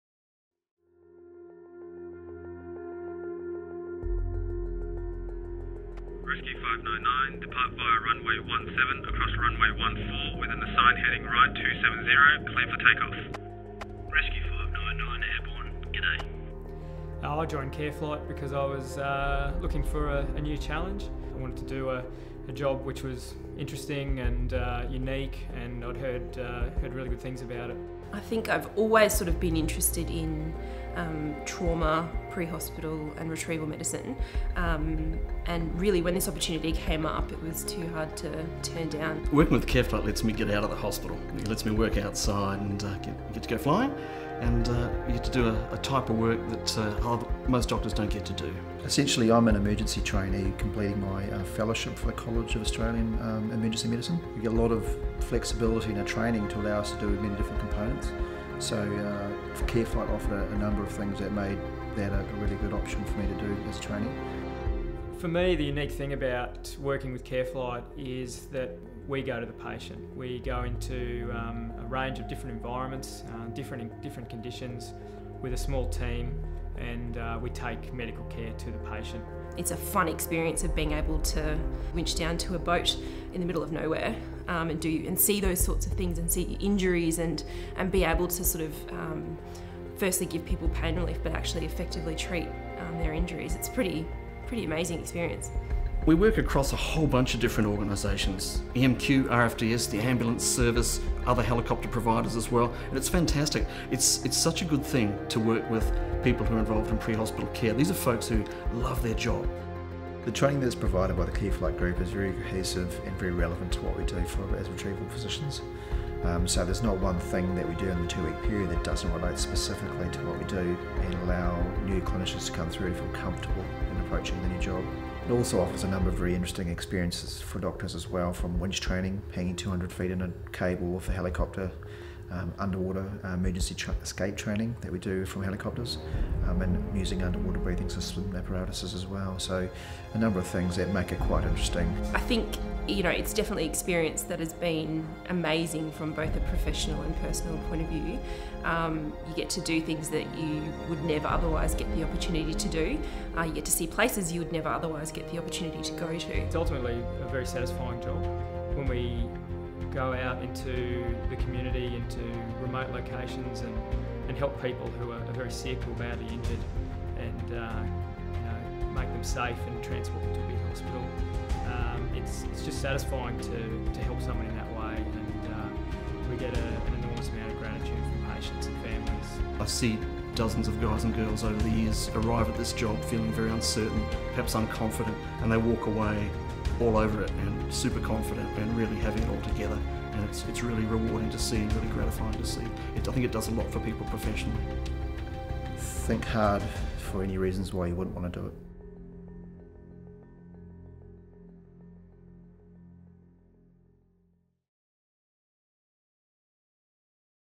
Your dream career as a LifeFlight doctor
Practicing flight doctors explain why working as a CareFlight (now LifeFlight) Registrar ticks all the boxes for their careers.

LifeeFlight recruits retrieval registrars for six and 12 month positions. Placements start in February and August.